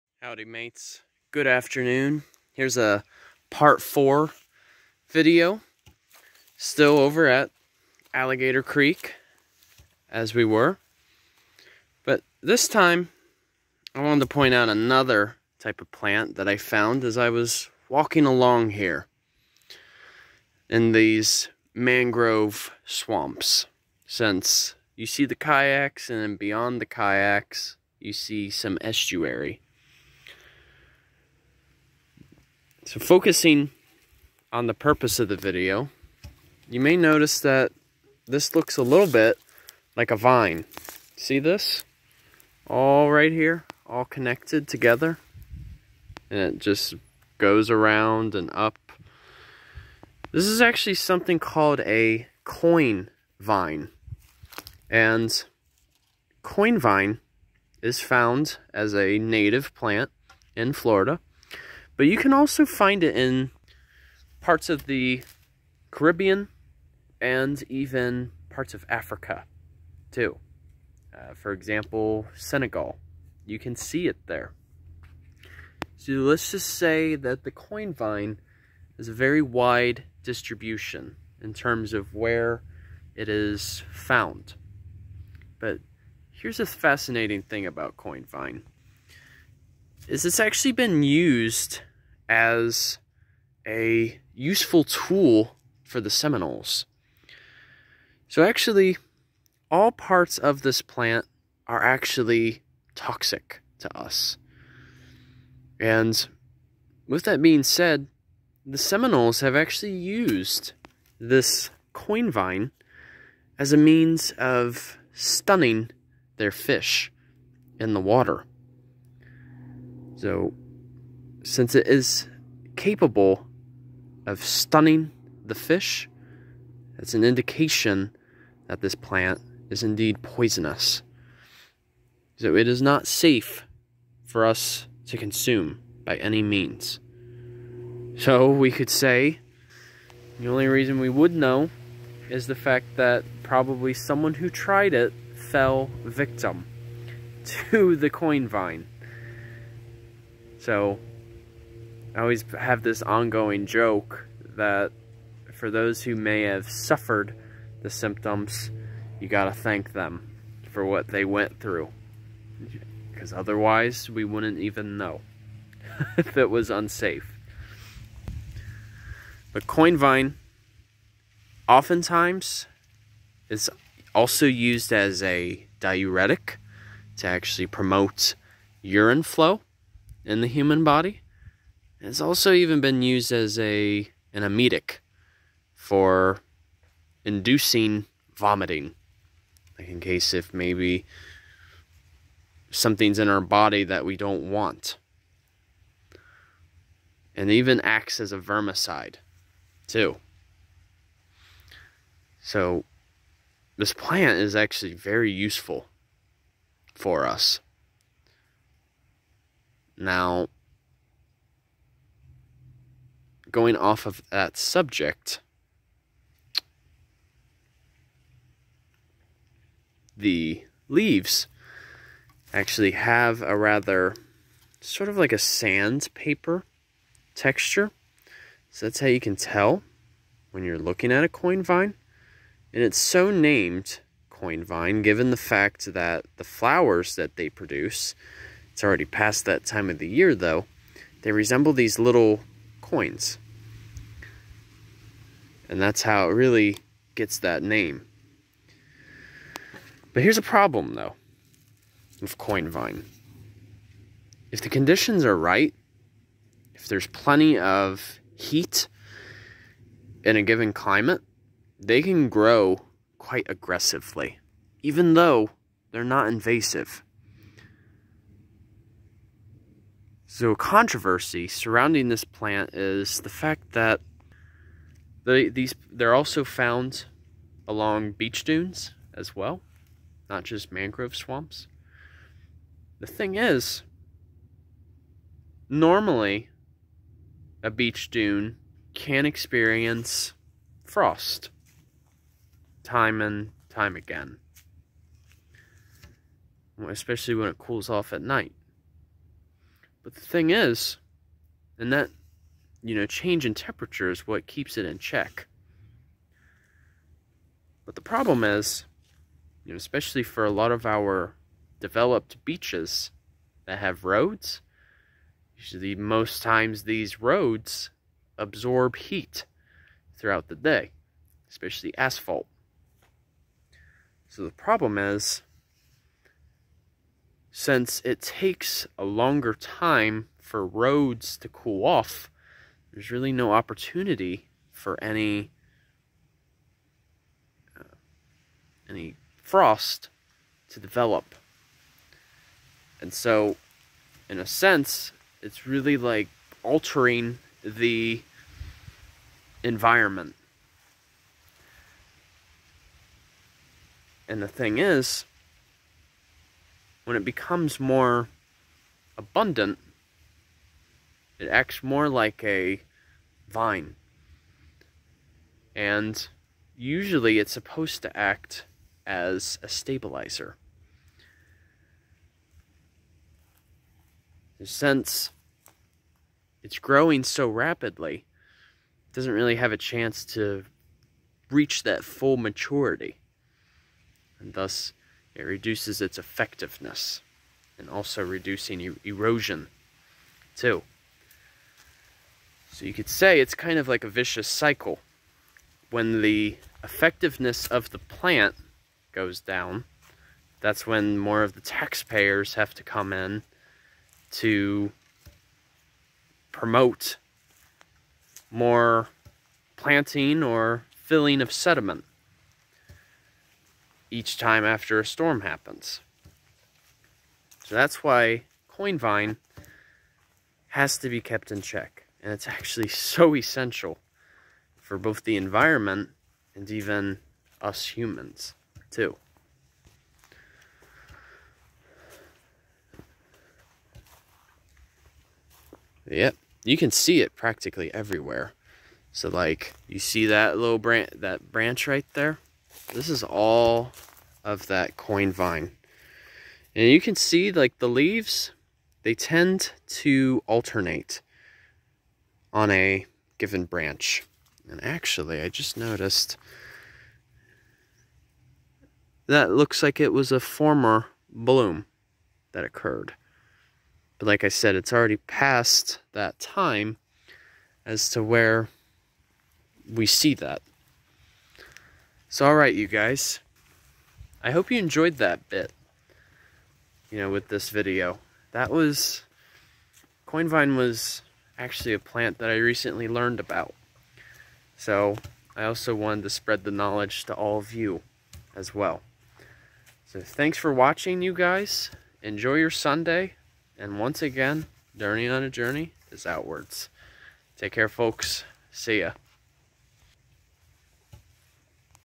The Coinvine
Video #4 of 5:
In which, I make a video on the Coin Vine that is a shrub and a legume. The plant essentially acts like a vine in Florida's ecosystem. The native plant has many uses for people. The Seminole tribe used it for stunning their fish due to its anesthetic properties (rotenone). It is not advisable for consumption as it is toxic. Not only that, but is acts as a diuretic for proper urine flow and an emetic to induce vomiting.

When too much heat is present, the plant acts like a vine and will prevent other native vegetation from growing by spreading. When that happens, the beaches will lose their ability to have stabilization. Much of this happens because of development, especially from roads.